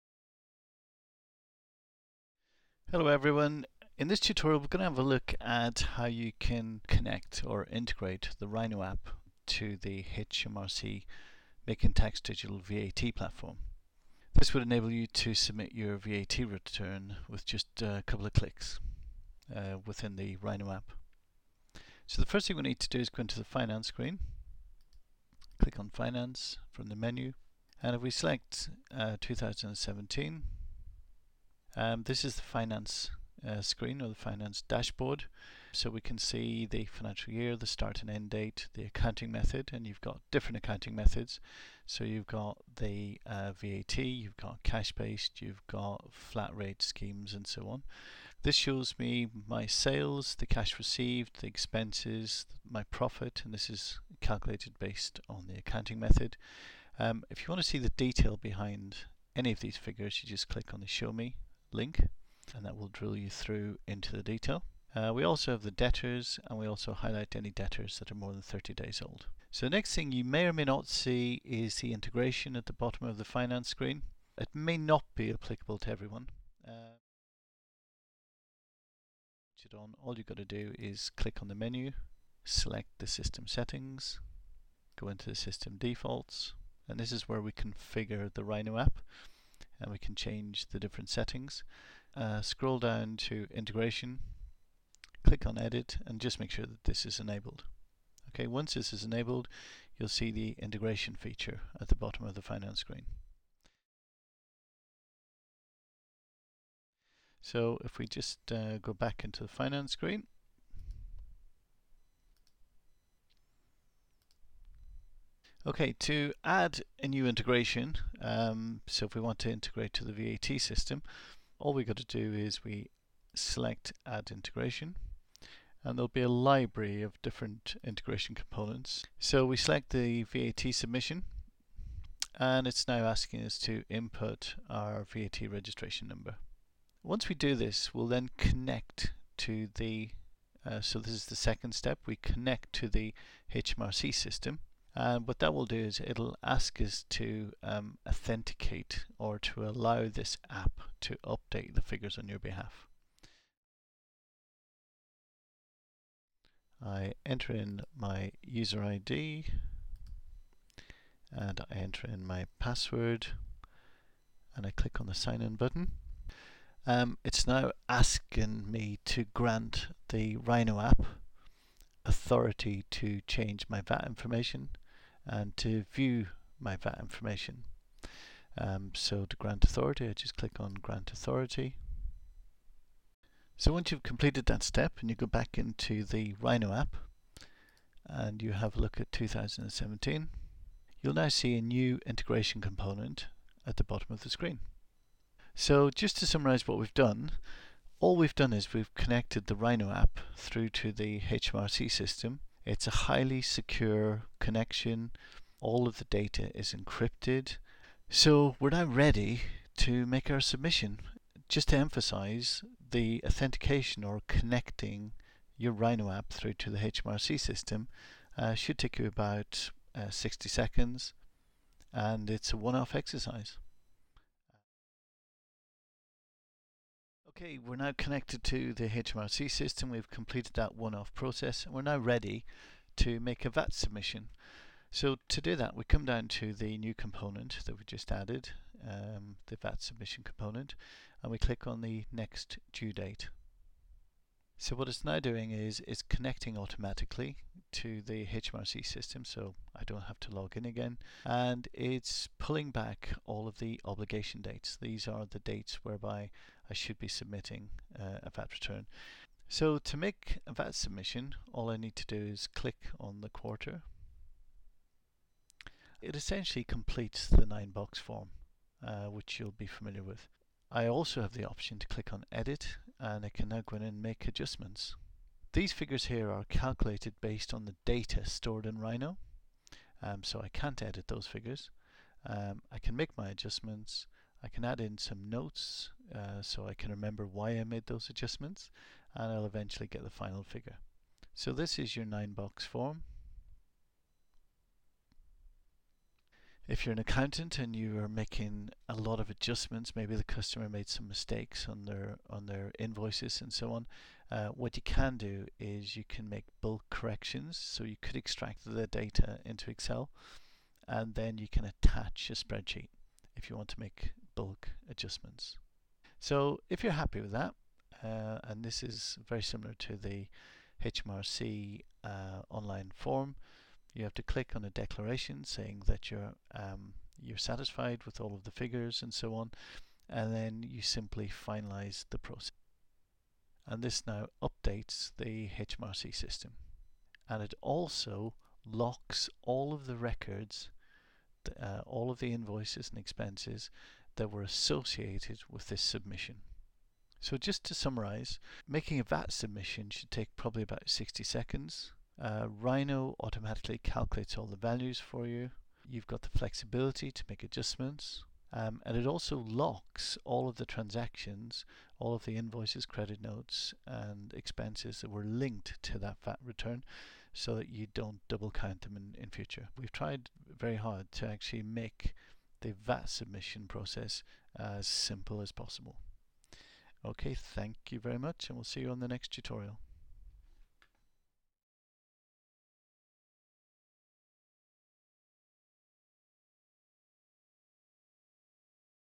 Rhino - Making Tax Digital (MTD) for VAT easy
This tutorial illustrated how to use the Rhino Small Business App to make a VAT Submission to the new HMRC Making Tax Digital (MTD) Platform in the UK.